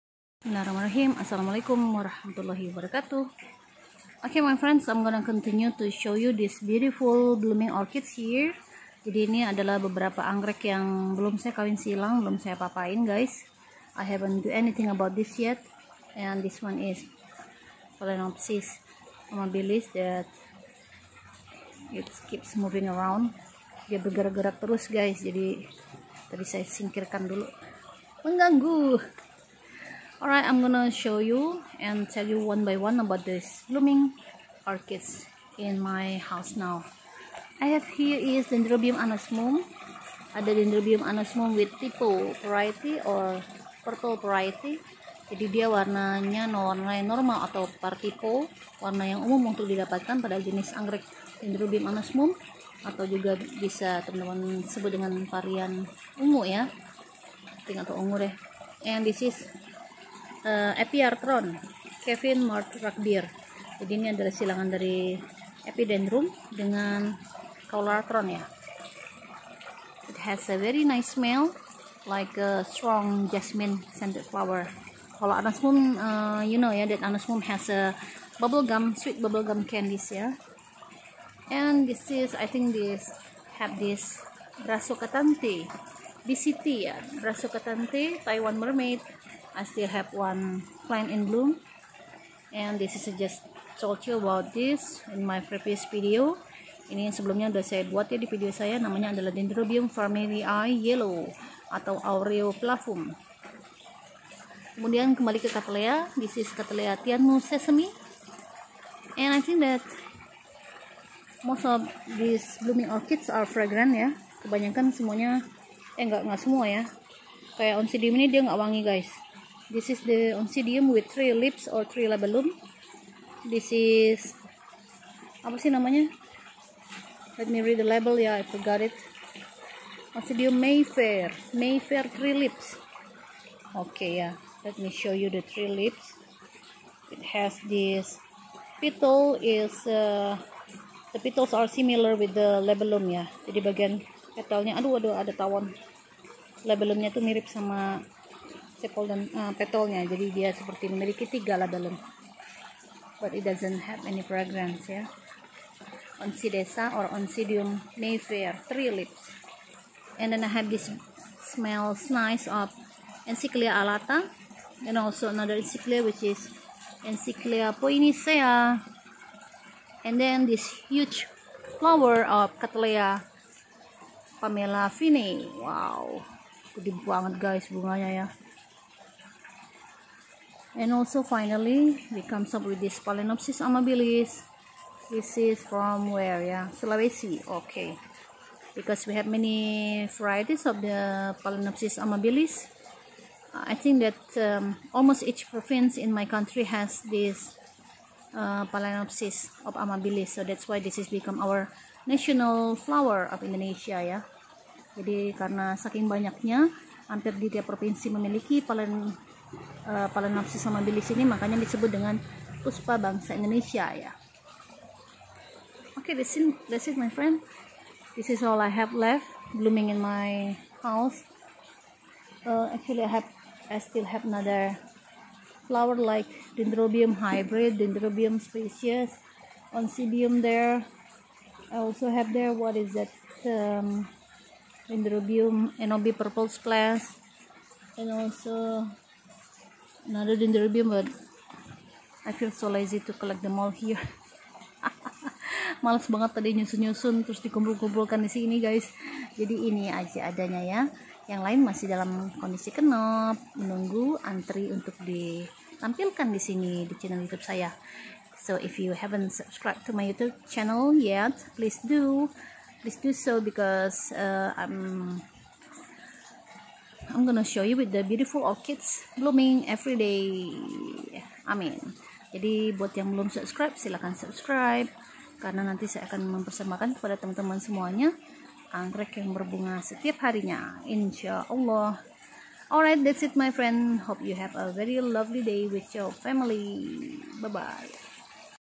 Orchids Fragrant Gallery
Orchids in bloom and dominating with fragrance flowers. Awesome fresh air ❤️